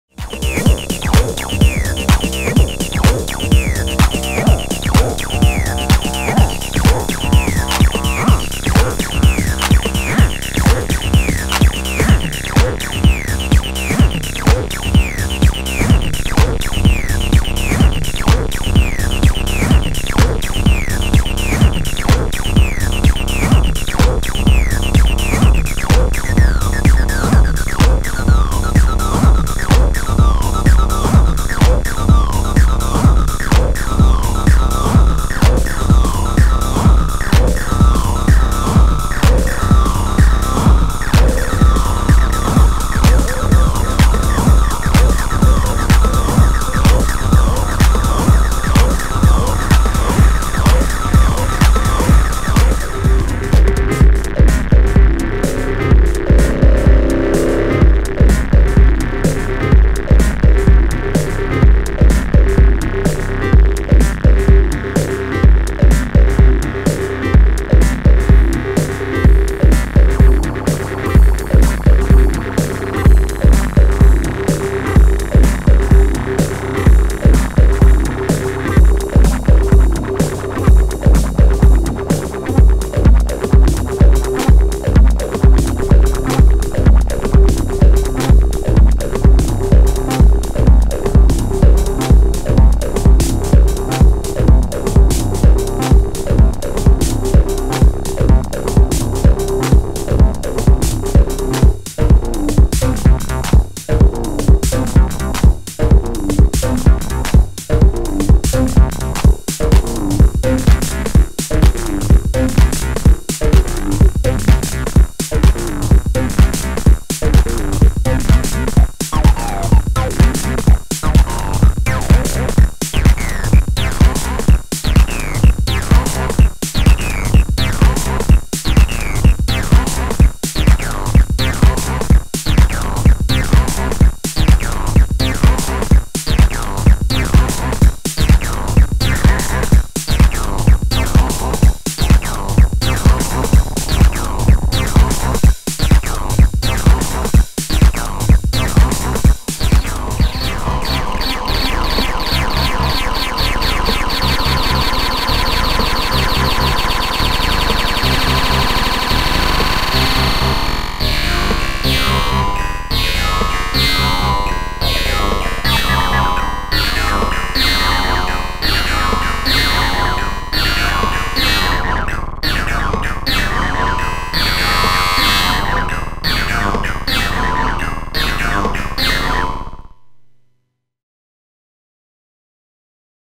Roland TR-09 TB-03 vs. MeeBlip triode
Live jam with Roland Boutique TR-09, TB-03, and the MeeBlip triode - nothing else, no effects. A little tiny live techno rig. Thanks to Nick from Roland for joining us on this one.